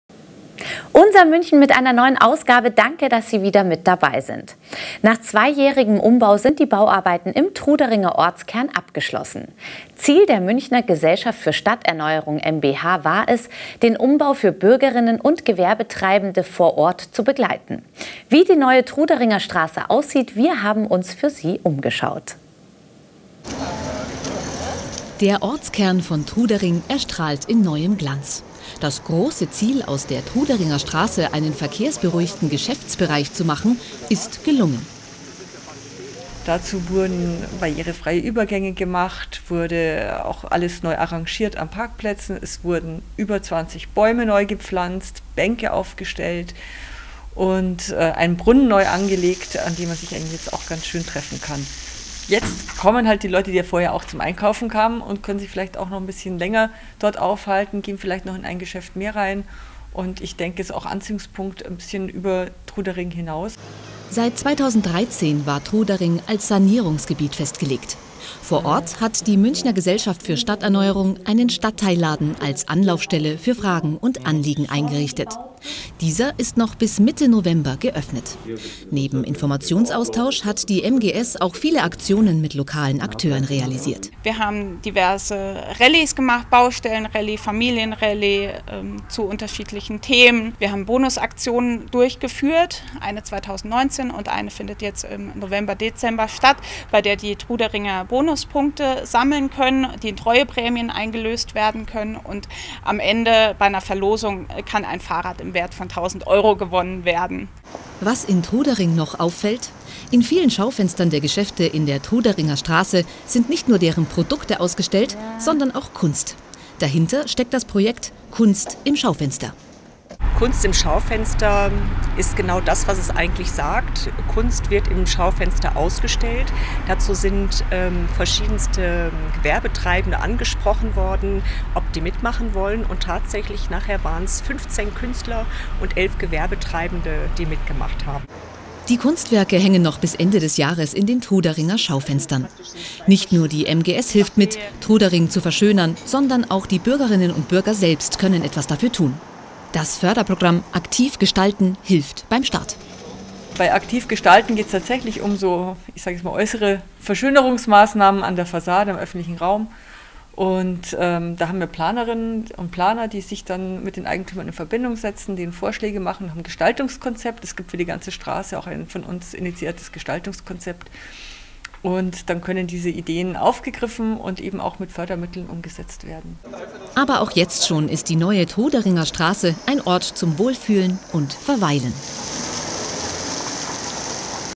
Bauarbeiten im Truderinger Ortskern sind abgeschlossen
Ziel der Münchner Gesellschaft für Stadterneuerung mbH war es, den Umbau für Bürger*innen und Gewerbetreibende vor Ort zu begleiten.

Wie die neue Truderingerstraße aussieht sehen Sie hier.